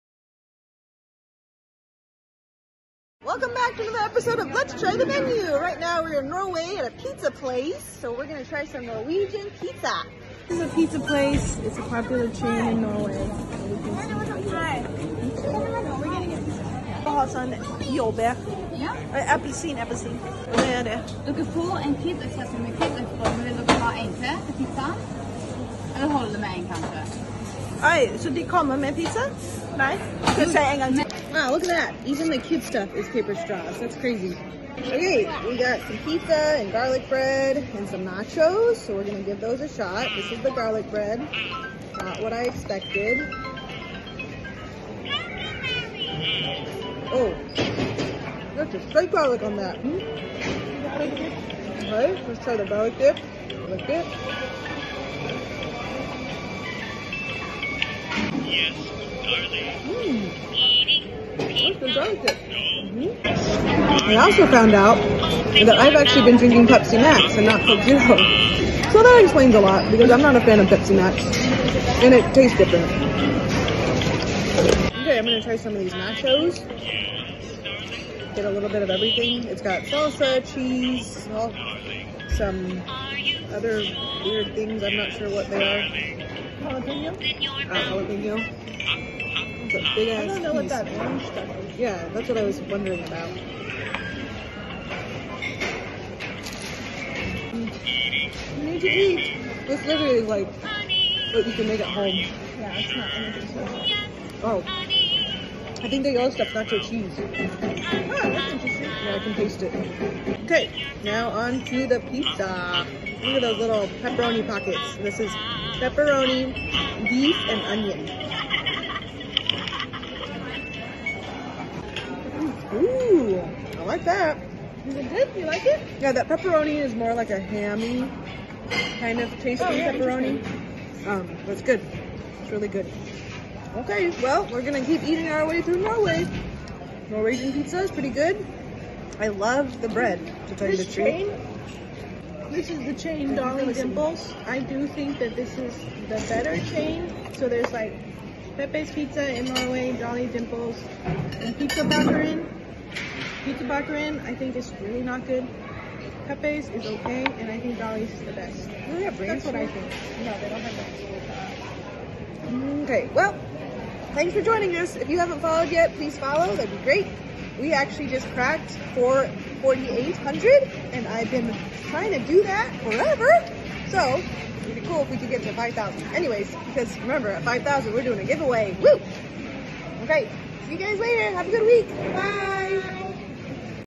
Food vlog 10! Let’s try the Menu! Delicious NORWEGIAN PIZZA
Just two picky friends and a bottomless husband, ordering other people's food. Give us a challenge! New episodes once a week.

This time we are travelling to Norway and trying new foods. Come and enjoy the taste with me!

Food Vlog 10
Let's enjoy some Norwegian Pizza.